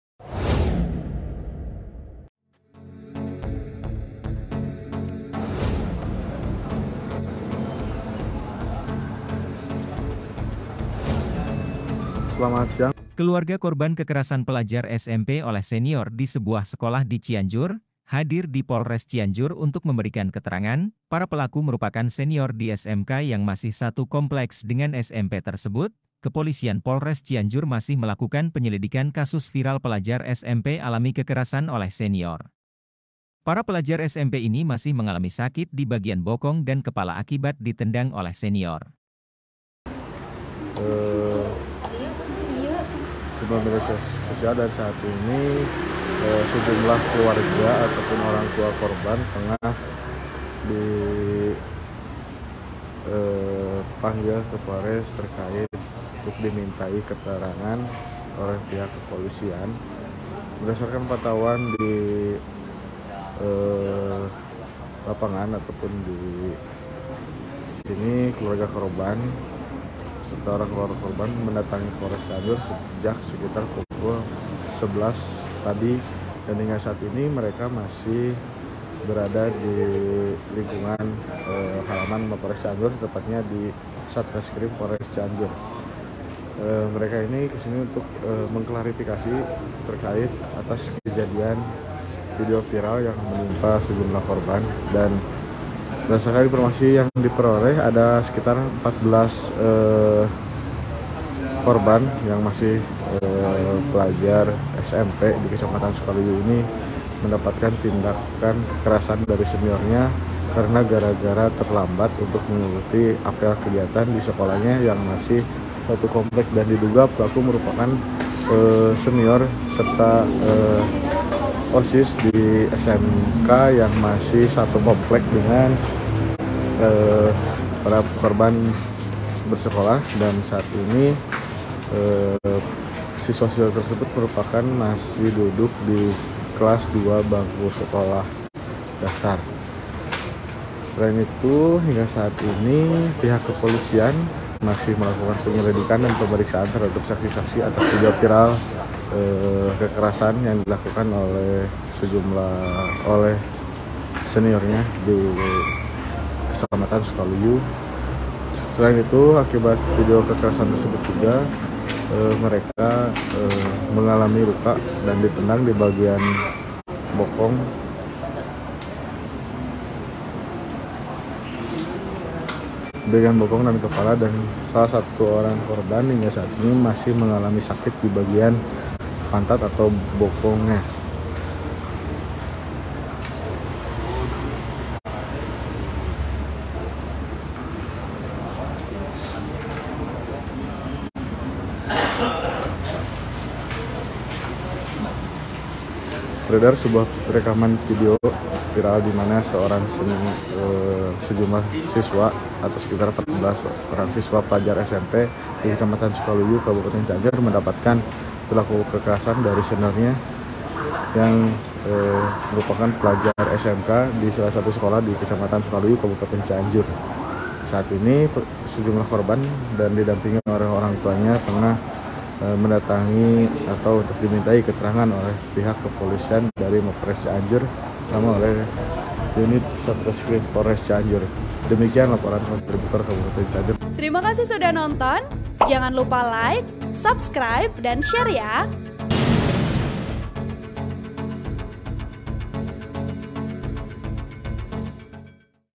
Siswa SMP Dihajar Senior di Cianjur Sakit di Bokong dan Kepala, Ortu Datangi Polisi
Sakit di Bokong dan Kepala

TRIBUNJABAR.ID, CIANJUR - Keluarga korban kekerasan pelajar SMP oleh senior di sebuah sekolah di Cianjur, hadir di Polres Cianjur untuk memberikan keterangan.

Para pelaku merupakan senior di SMK yang masih satu kompleks dengan SMP tersebut.

Kepolisian Polres Cianjur masih melakukan penyelidikan kasus viral pelajar SMP alami kekerasan oleh senior.

Para pelajar SMP ini masih mengalami sakit di bagian bokong dan kepala akibat ditendang oleh senior.*

LIVE - Keluarga Korban Kekerasan Seniornya Datangi Polres Cianjur.

Laporan Fauzi Noviandi | Video Editor: Edwin Tk